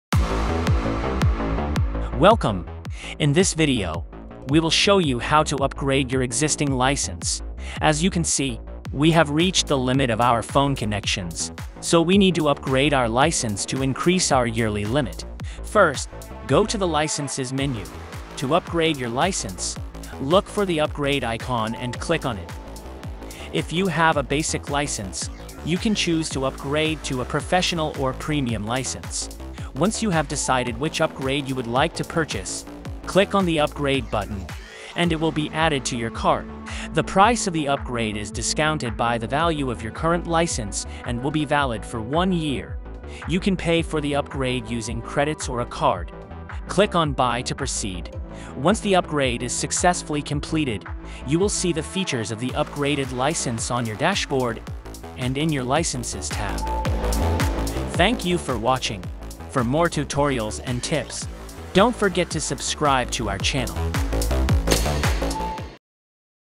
Upgrading your ChimeraTool license
In this video, we will show you how to upgrade your ChimeraTool licence